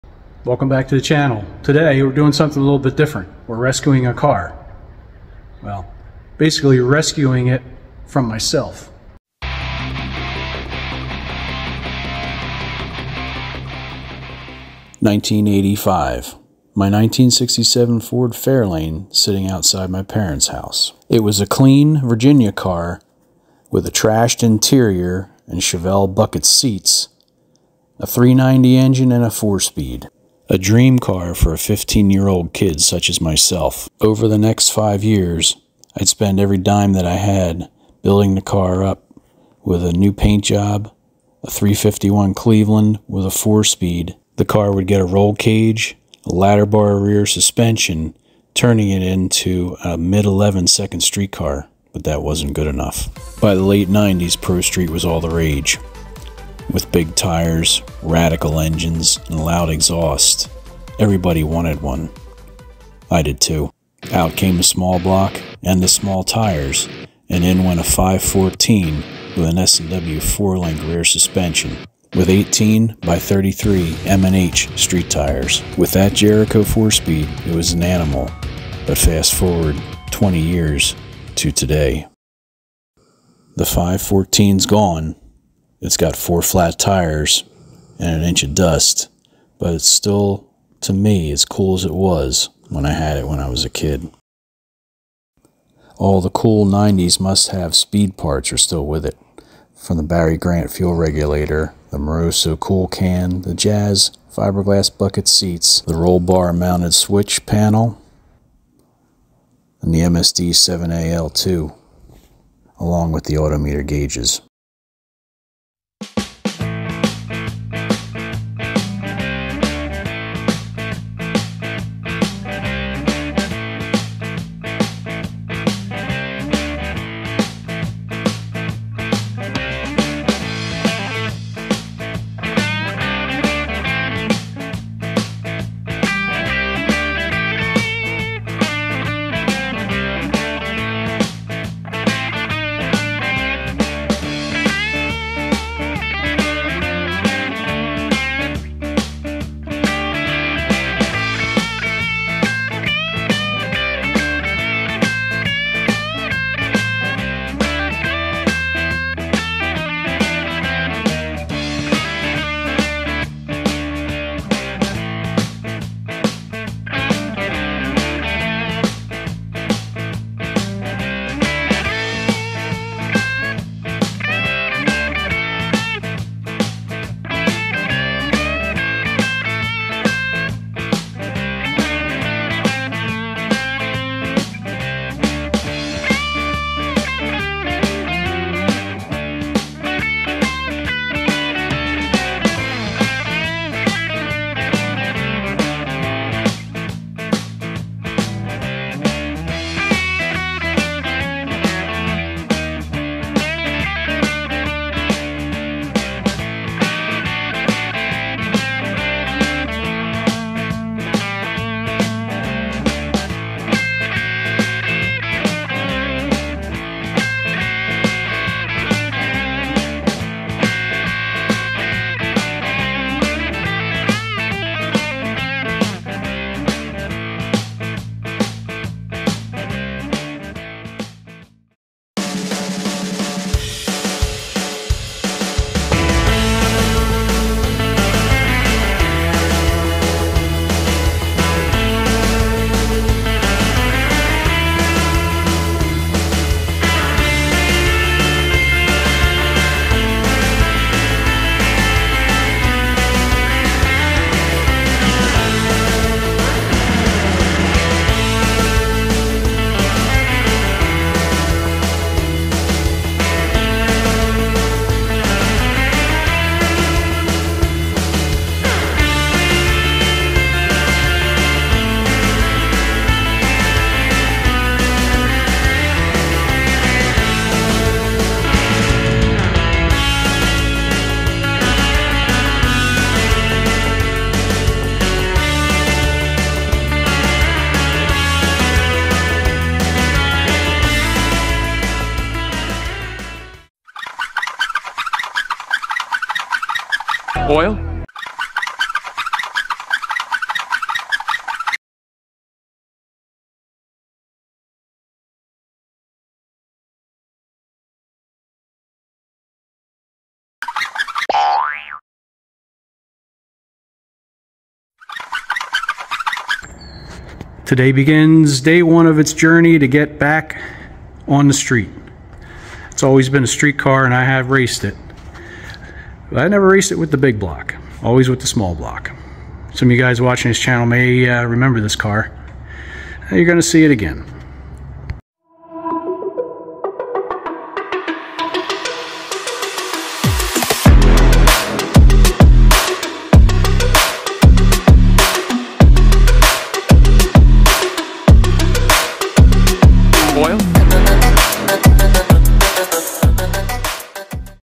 1967 Ford Fairlane Pro Street RESCUE
Pulling my 67 Pro Street Fairlane out of a 20+ year storage